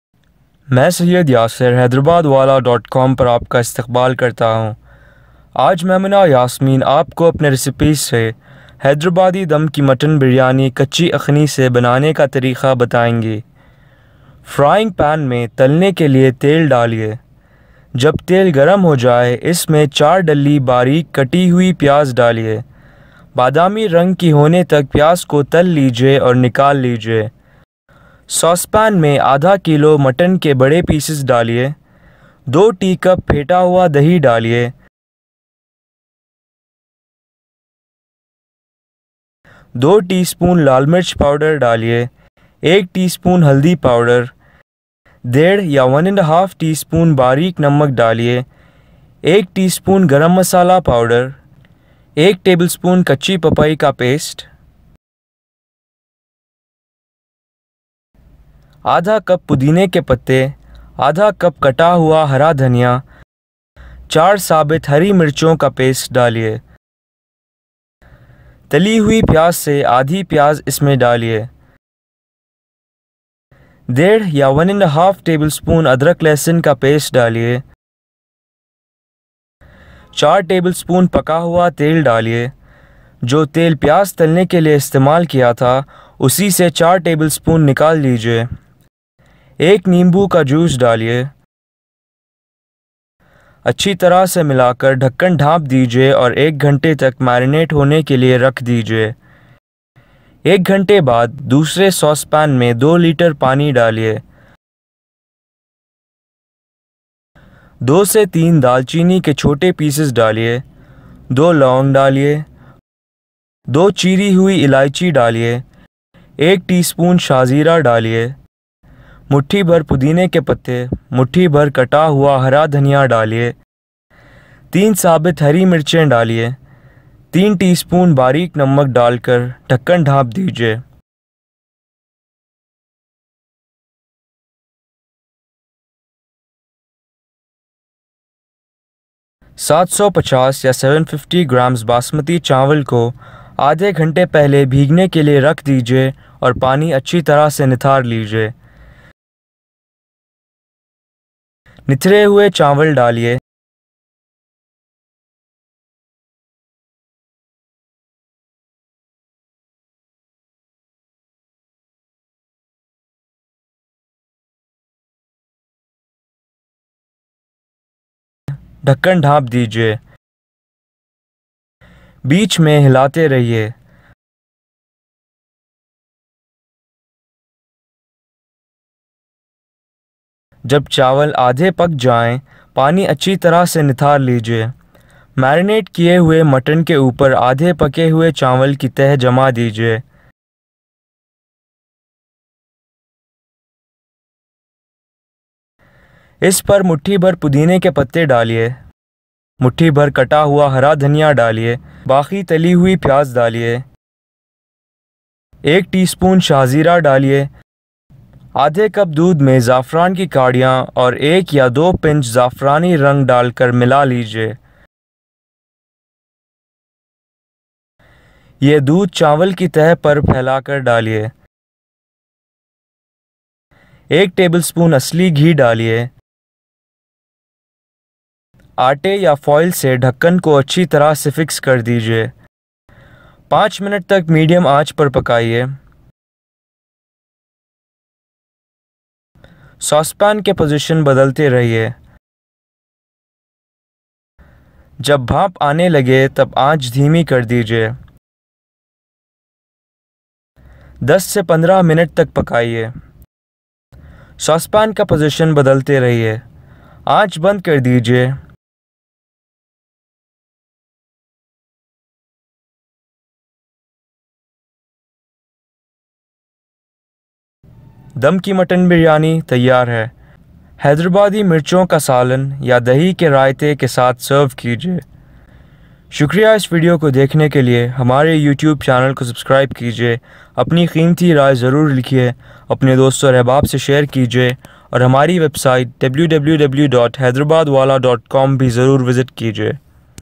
Hyderabadi Mutton Biryani with Kacchi Aqni Recipe Video in Hindi/ Urdu
Hyderabadi Dum Ki Mutton Biryani Kachhi Aqni from Maimoona Yasmeen’s Recipes for 4 to 5 servings

Ingredients:

Pyaaz (onions) -  4
Talne ke liye tel (Oil for frying)

Procedure:

1. Frying pan mein talne ke liye tel daliye.

2. Jab tel garam ho jaaye, is mein bareek kati hui pyaz daliye. Badami rang ki hone tak pyaz ko tal lijiye aur nikal lijiye.

For mutton marination (Aqni banane ke liye):

Ingredients:

Mutton (Big Pieces):  ½ kg
Dahi (Curd): 2 cups
Adrak lehsan ka paste (ginger garlic paste): 1 ½ teaspoon
Red Chilli Powder: 2 teaspoons
Turmeric Powder: 1 teaspoon
Salt: 1 ½ teaspoons
Garam masala powder (spices): 1 teaspoon
Kachhi papai ka paste (raw papaya paste): 1 tablespoon
Pudine ke patte (mint leaves): ½ cup
Kata hua hara dhaniya (chopped coriander): ½ cup
Sabith hari mirchein (whole green chillies): 4
Paka hua tel (Cooked oil): 4 tablespoons (pyaaz talne ke liye istemaal kiye hue tel se nikal lijiye)
Nimbu (lemon): 1
Tai hui pyaaz (fried onions): aadhi (half of the fried onions)

Procedure:

1. Saucepan mein mutton ke bade pieces daliye.

2. Pheta hua dahi is gosht mein daliye. Adrak lehsan ka paste, lal mirch powder daliye, haldi powder, bareek namak daliye, garam masala powder, kachhi papai ka paste daliye, pudine ke patte, kata hua hara dhaniya, hari mirchon ka paste daliye.

3. Tali hui pyaaz se aadhi pyaaz is mein daliye. Paka hua tel daliye (Jo tel pyaz talne ke liye istemaal kiya tha, usi se chaar tablespoon nikal lijiye).

4. Nimbu ka ras daliye. Achhi tarah se milakar dhakkan dhak dijiye aur ek ghante tak  (1 hr) marinate hone ke liye rakh dijiye.
For cooking rice (chawal pakane ke liye):

Ingredients:

Basmati chawal (Basmati rice) – 750 grams
Paani (water): 2 litres
Dalchini (chhote pieces) (small sticks of cinnamon): 2 to 3
Lavang (black cloves): 2
Elaichi (cardamom): 2
Shahzeera (caraway seeds): 1 teaspoon
Pudine ke patte (mint leaves): mutthi bhar (a handful)
Kata hua hara dhaniya (chopped coriander): mutthi bhar (a handful)
Sabith hari mirchein (whole green chillies): 3
Bareek namak (salt): 3 teaspoons

Procedure:

1. Chawal ko aadhe ghante (1/2 hr) ke liye bheegne ke liye rakh dijiye.

2. Doosre saucepan mein do litre paani daliye.

3. Dalchini ke pieces daliye, lavang, cheeri hui elaichi, shah zeera daliye, pudine ke patte, kata hua hara dhaniya, hari mirchein aur bareek namak dalkar dhakkan dhaanp dijiye.

4. Jab paani mein ubaal aa jaye, chawal se paani achhi tarah se nithar kar, nithre hue chawal is ubalte paani mein daliye.

5. Dhakkan dhaanp dijiye. Beech mein hilate rahiye.

6. Jab chawal aadhe pak jaayen, pani acchhi tarah se nithar lijiye.

Dum dene ke liye (final preparation):

Ingredients:

Marinate kiya hua mutton
Aadhe pake hue chawal
Pudine ke patte (mint leaves): muthhi bhar (a handful)
Kata hua hara dhaniya (chopped coriander): muthhi bhar (a handful)
Baqi tali hui pyaz (remaining fried onions)
Doodh (milk): ½ cup
Zafraan (saffron sticks)
Zafrani rang (saffron colour): 2 pinches
Asli ghee (clarified butter): 1 tablespoon
Aata (dough) or foil for fixing the lid

Procedure:

1. Marinate kiye hue mutton ke upar aadhe pake hue chawal ki theh (layer) jama dijiye.

2. Is par pudine ke patte daliye, kata hua hara dhaniya, baqi tali hui pyaz, shahzeera daliye.

3. Doodh mein zafran ki kadiyan aur zafrani rang dalkar mila lijiye. Yeh doodh chawal par phailakar daliye.

4. Asli ghee daliye.

5. Aate (dough) ya foil se dhakkan ko achhi tarah se fix kar dijiye.

6. Paanch minute (5 min) tak medium aanch par pakaiyye.

7. Saucepan ka position badalte rahiye. Jab bhaanp aane lage tab aanch dheemi kar dijiye.

8. Das se pandrah minute (10 to 15 min) tak pakaiyye.

9. Saucepan ka position badalte rahiye. Aanch band kar dijiye.

Dum ki Mutton biryani tayyar hai. Hyderabadi mirchon ka salan ya dahi ke raithe ke saath serve kiiye.